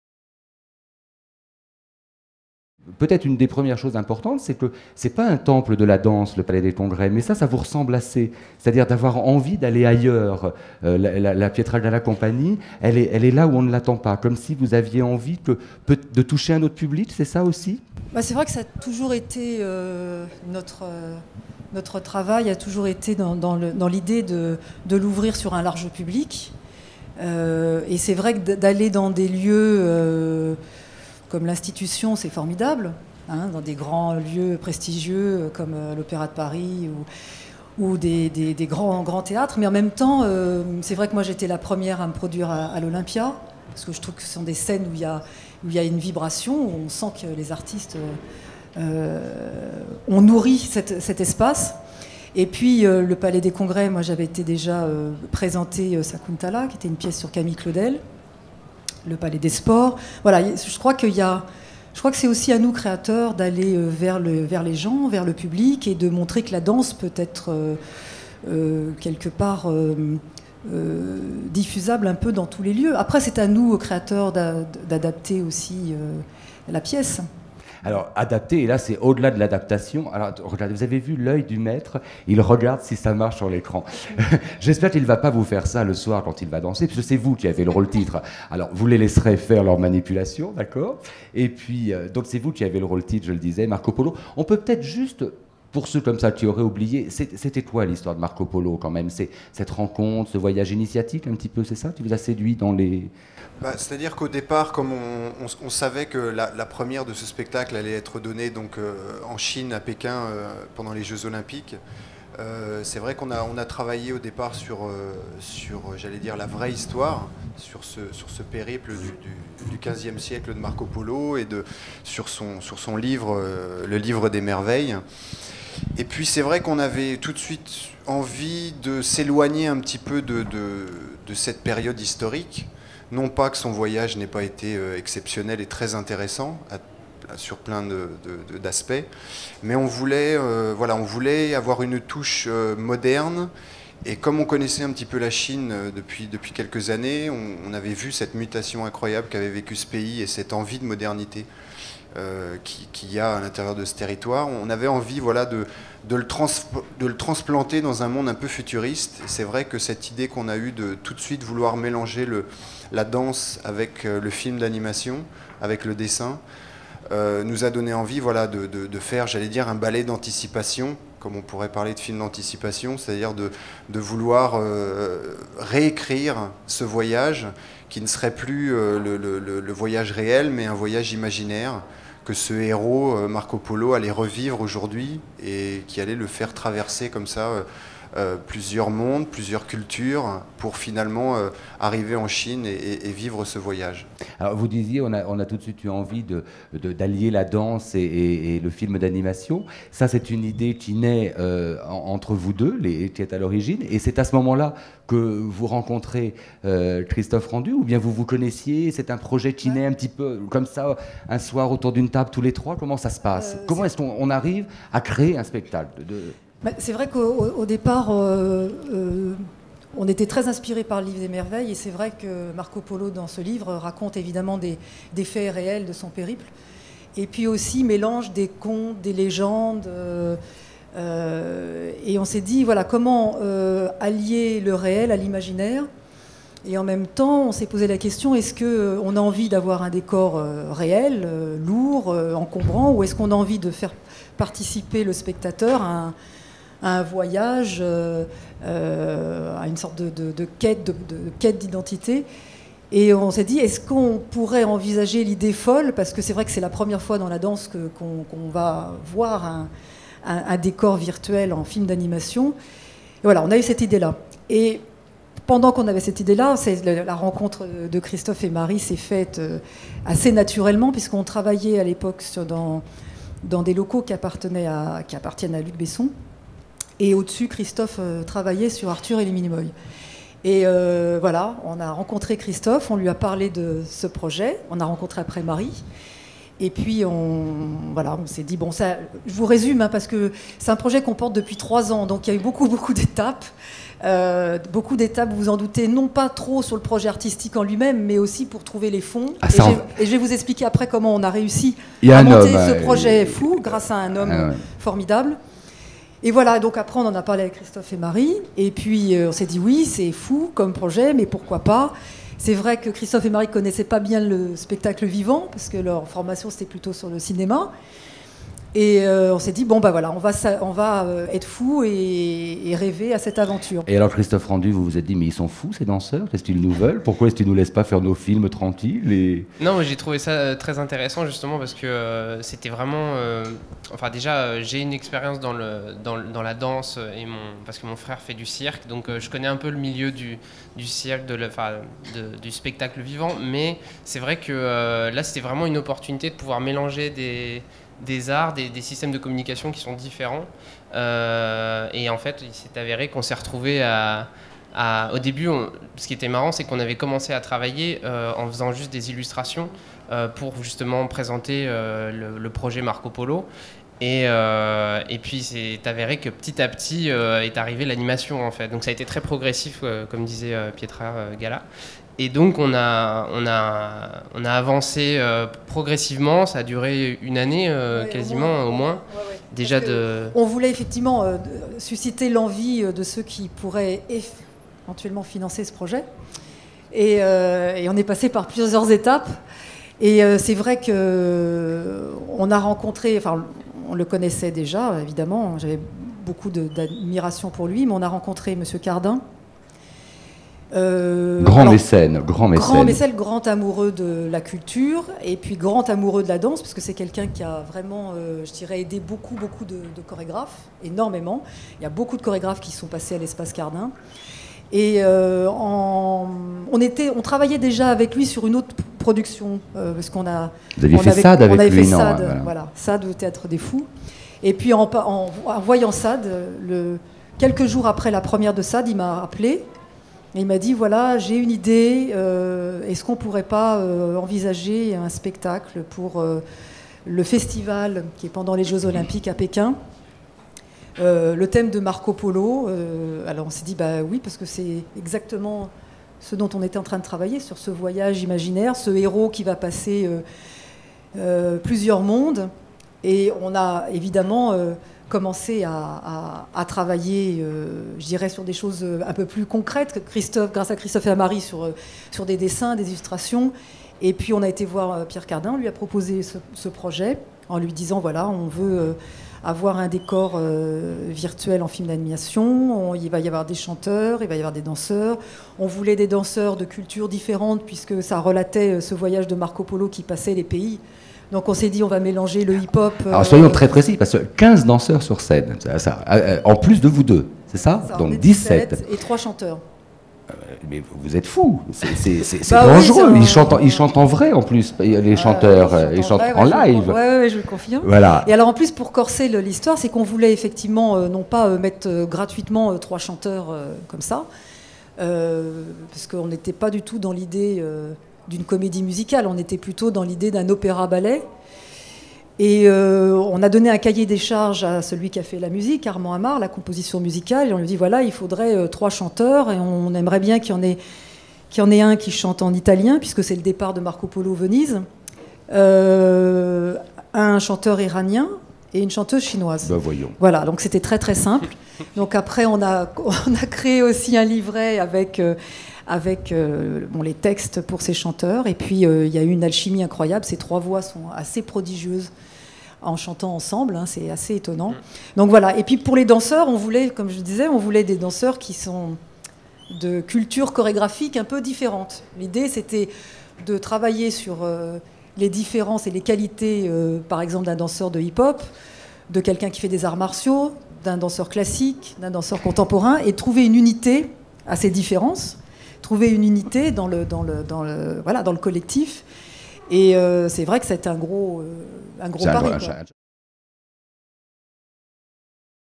Rencontre "Marco Polo" avec Marie-Claude Pietragalla (1/2)
Rencontre avec Marie-Claude Pietragalla et Julien Derouault à propos du spectacle "Marco Polo", le 16 janvier 2009 à la Fnac Montparnasse.